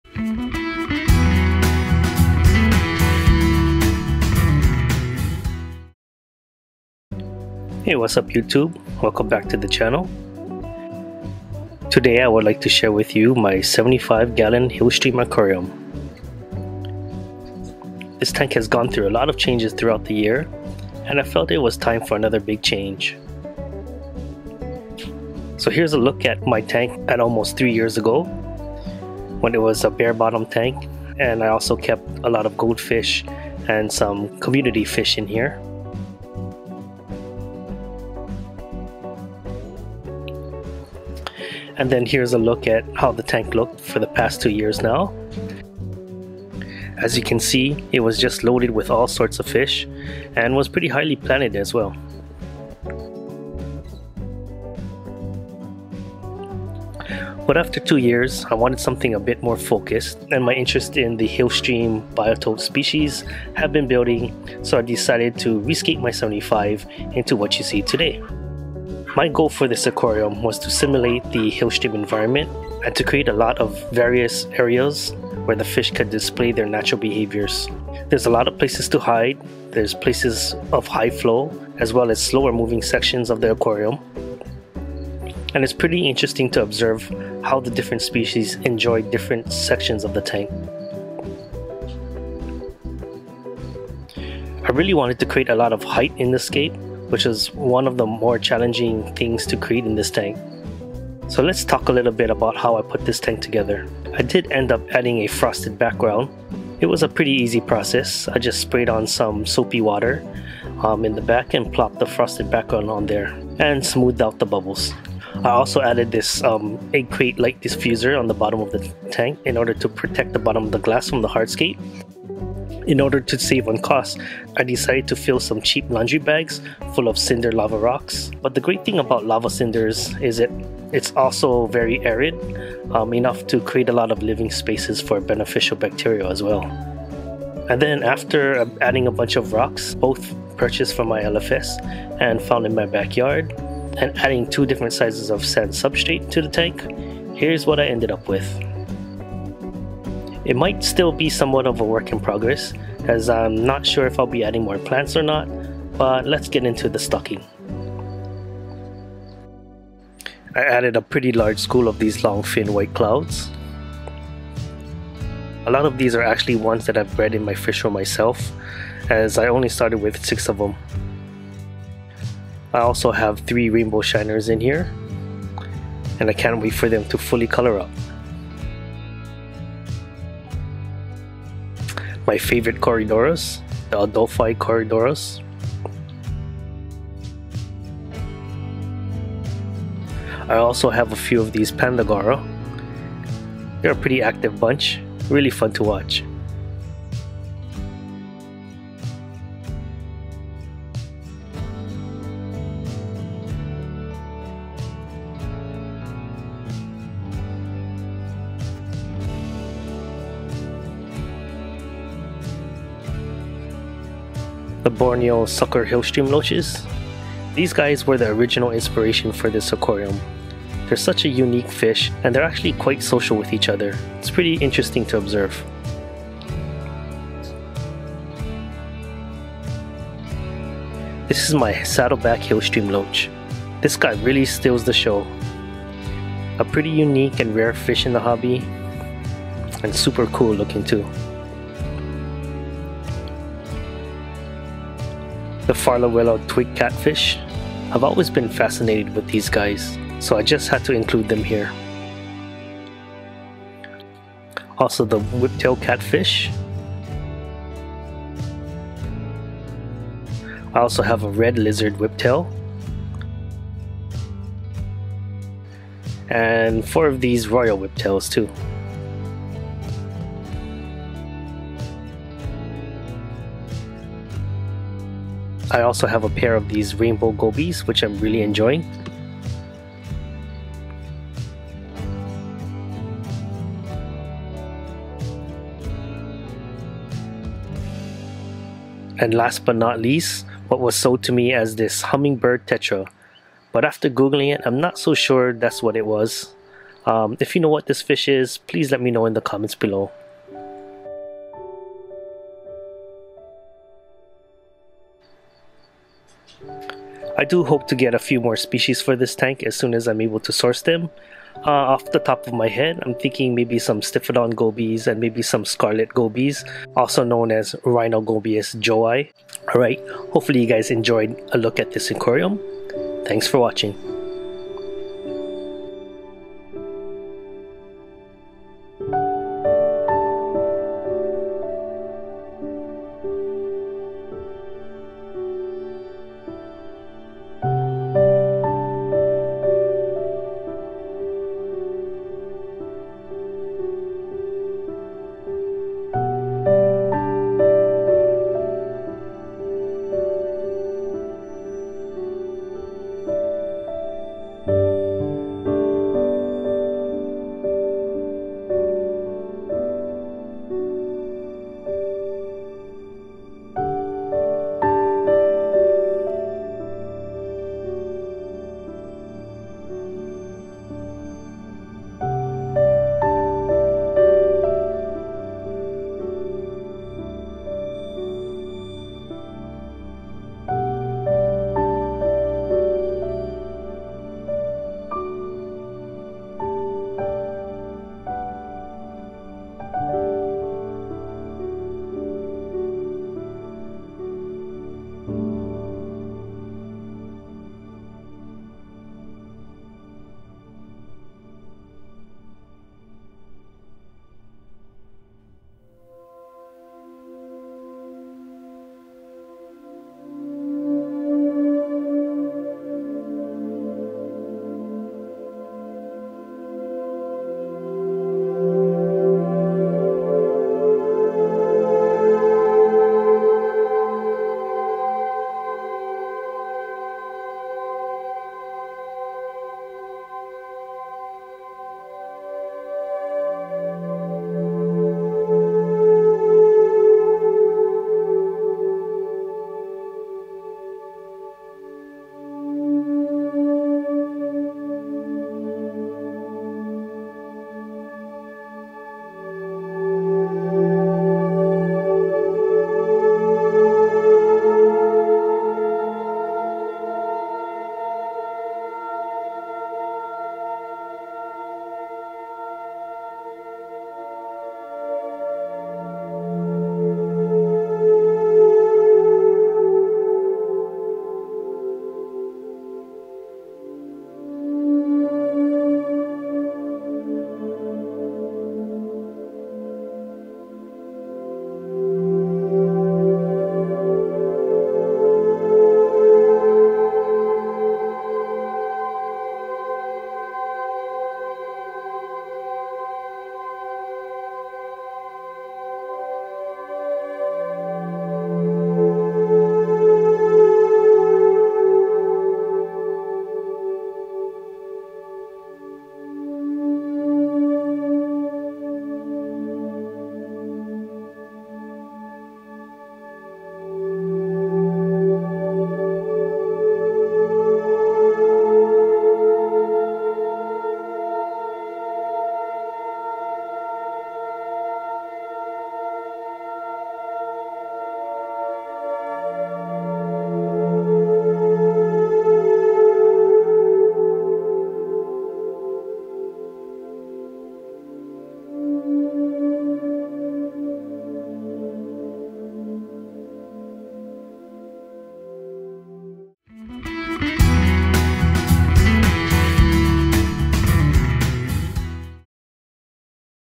75 Gallon Hillstream Aquarium
I decided to convert my 75 Gallon Aquarium into a Hillstream tank.  Come see how I put it together and what I decided to stock in it.